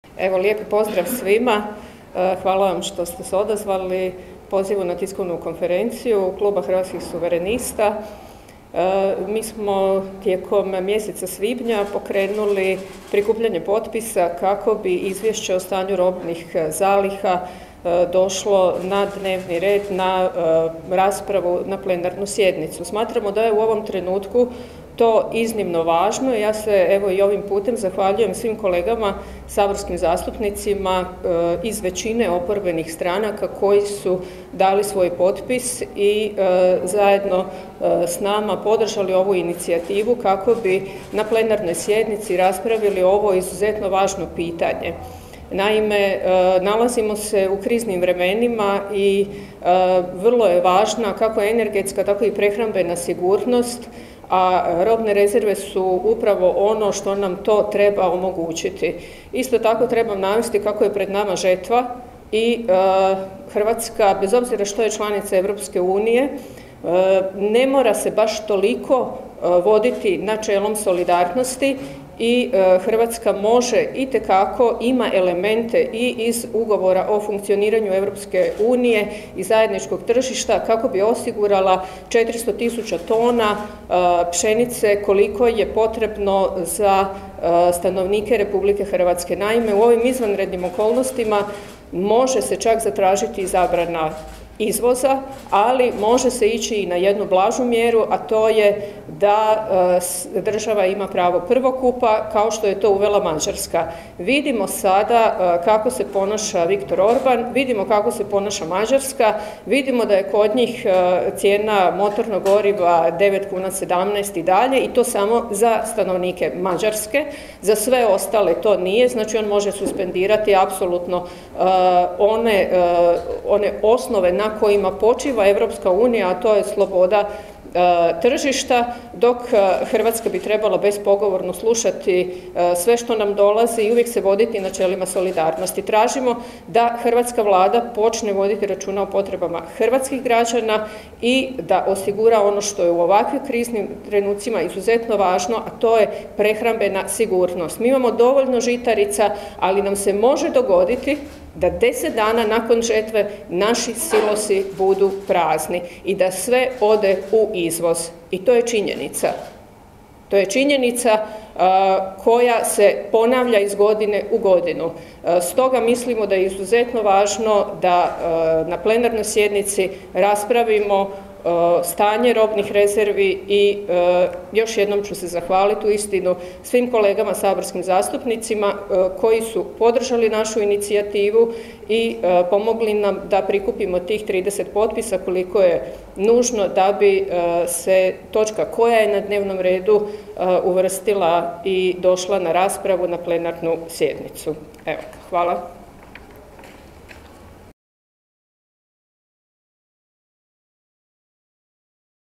Vesna Vučemilović o robnim zalihama
Na konferenciji za medije Hrvatskih suvrenista održanoj u Saboru Vesna Vučemilović govorila je o robnim zalihama.